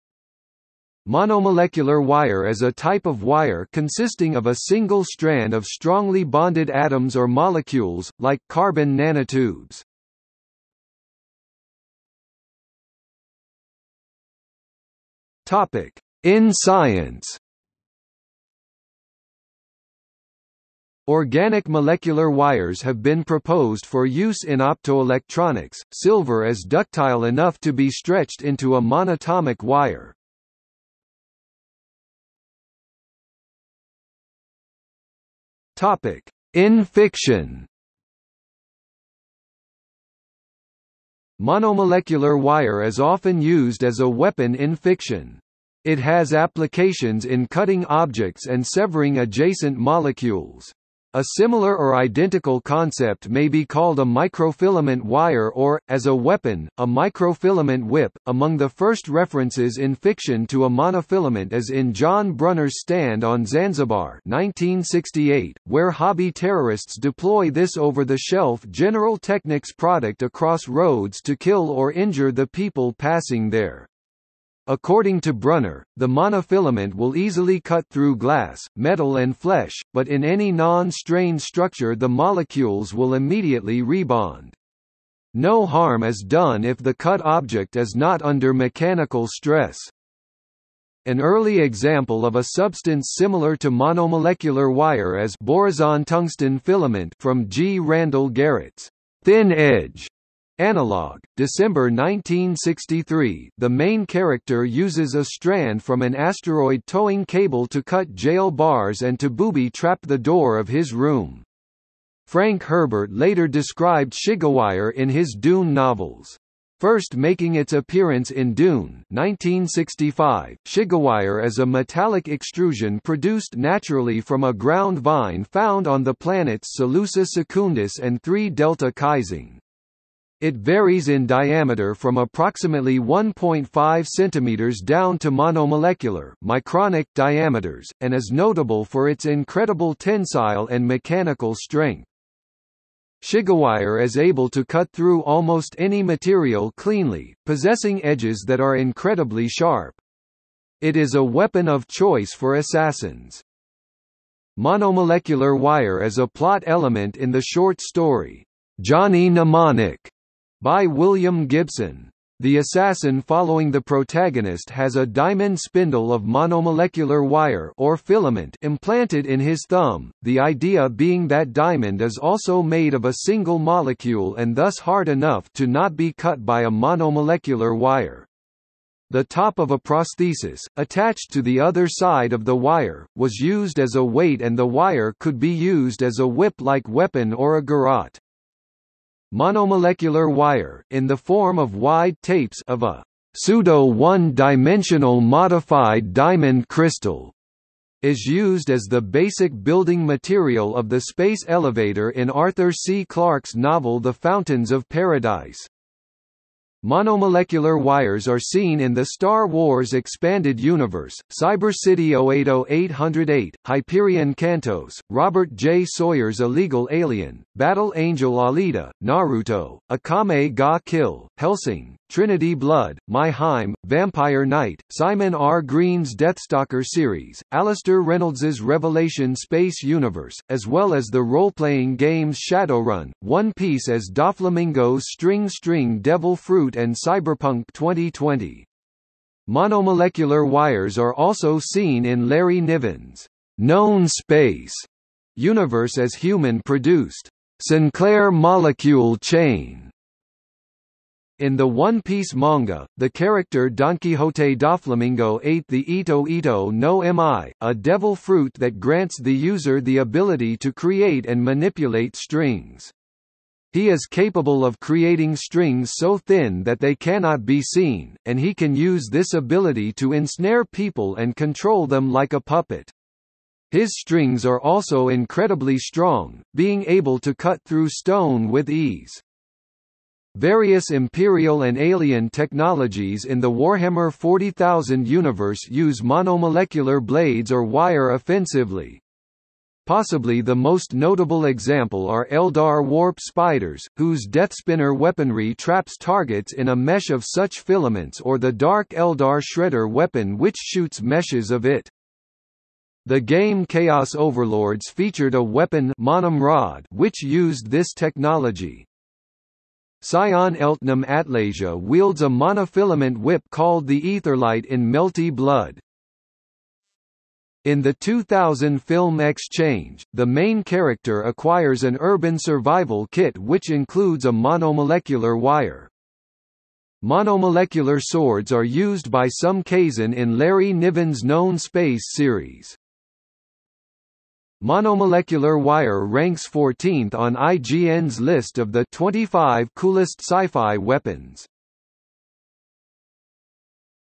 Monofilament (science fiction) | Wikipedia audio article
00:00:11 In science
00:00:32 In fiction

- Socrates

SUMMARY
Monomolecular wire is a type of wire consisting of a single strand of strongly bonded atoms or molecules, like carbon nanotubes.